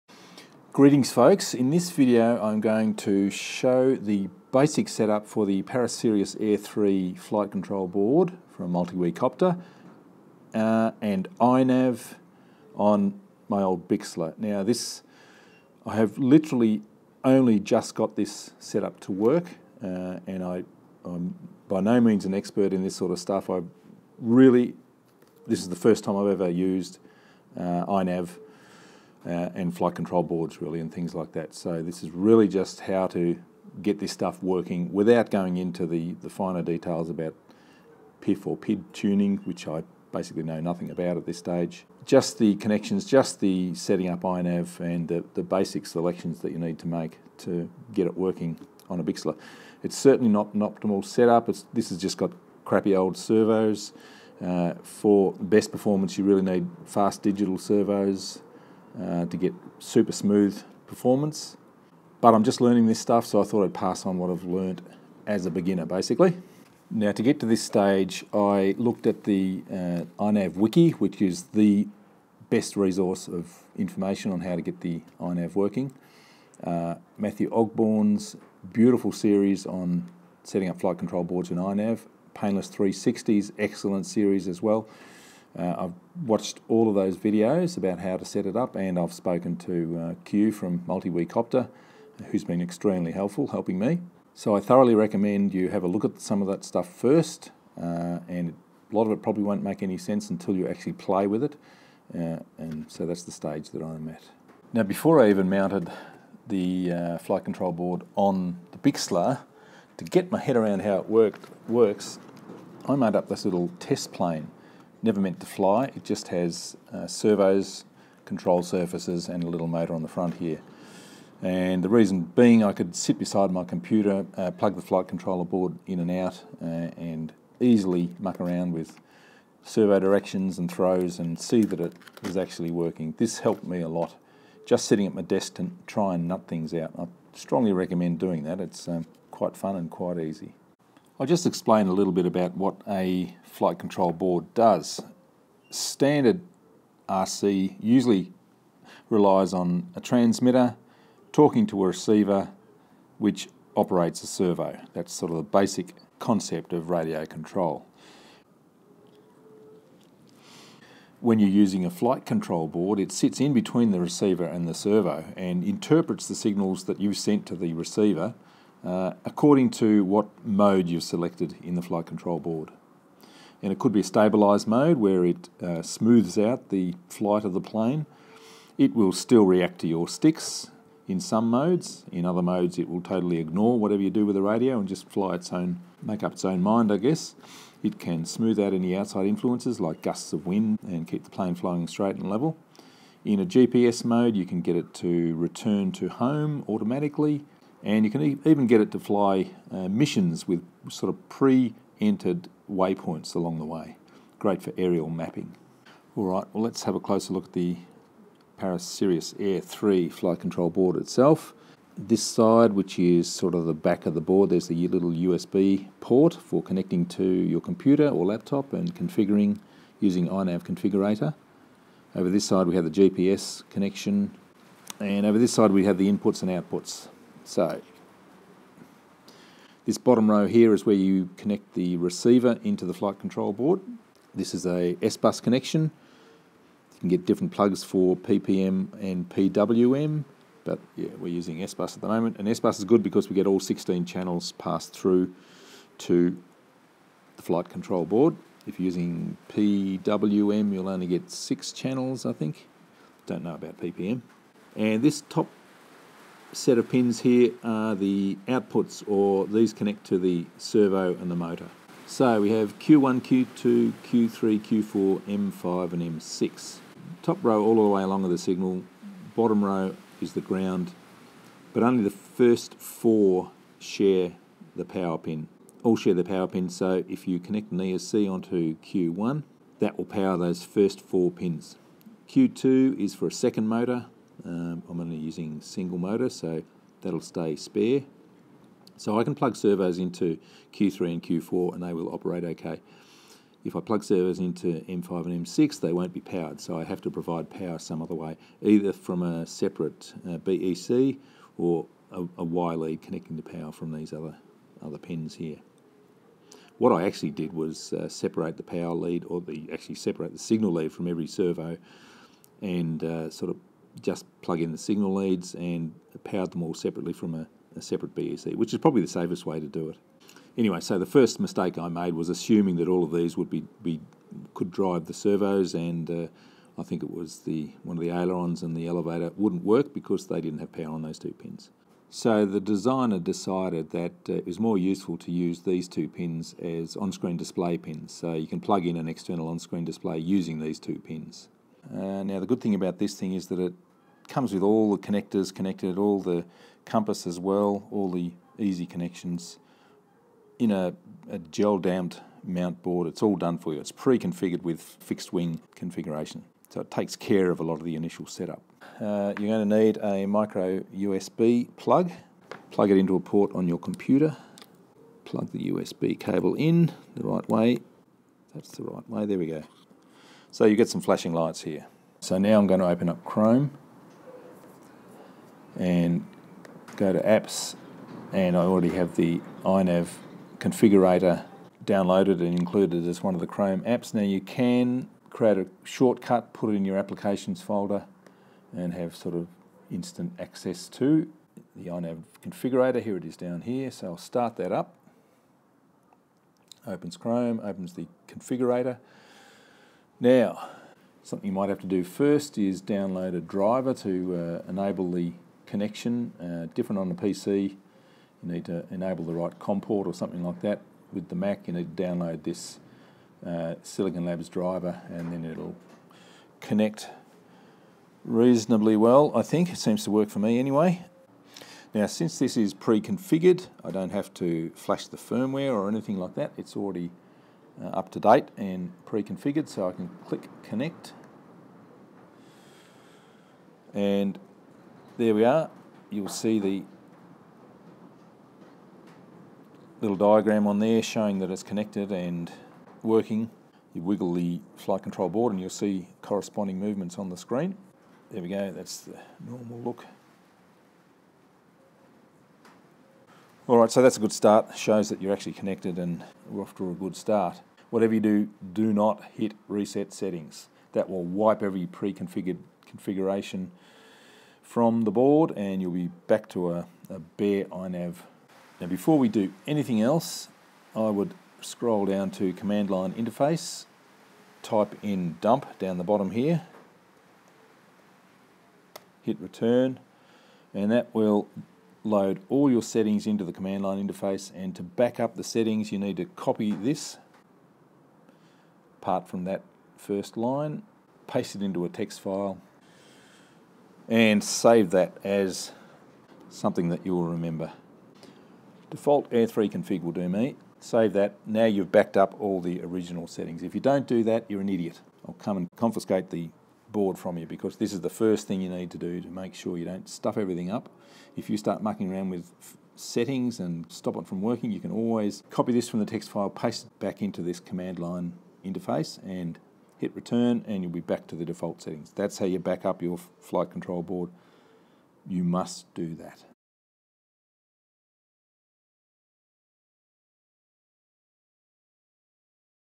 Bixler 2 AIR3 iNav Part 1
Part 1 of a 3 part beginner's series on the Paris Sirius AIR3 flight control board using iNav on a Bixler 2
Introduction, overview of the board and connection to iNav Configurator.